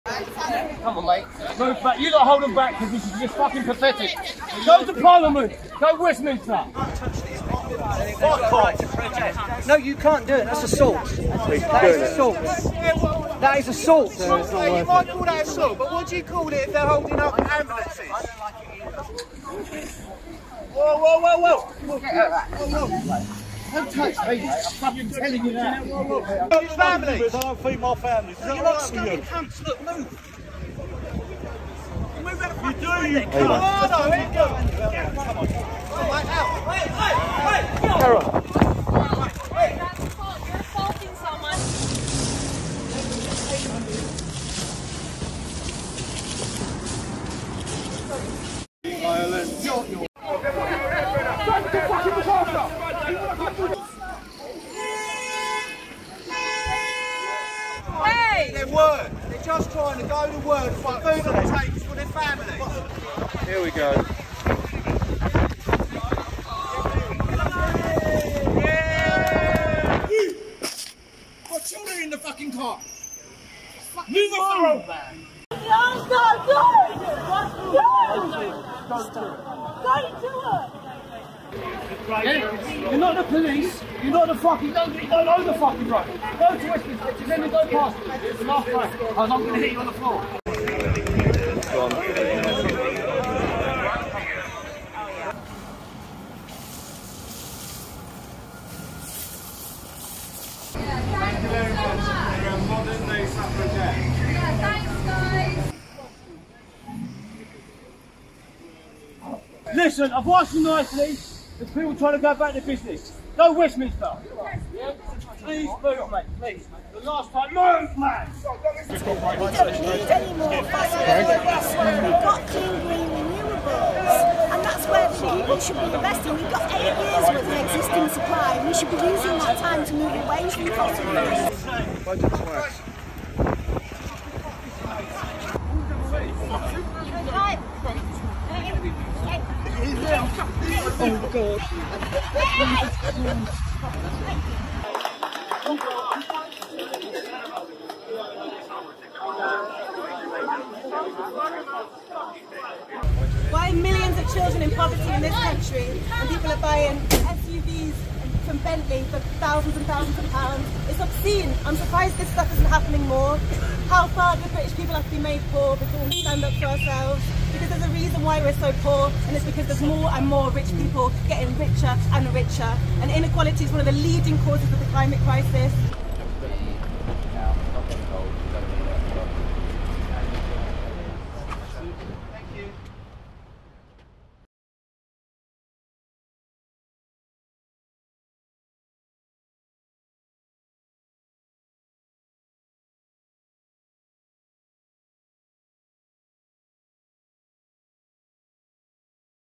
Public violently drag Just Stop Oil protesters off street
Watch as members of the public aggressively drag Just Stop Oil protesters from London roads.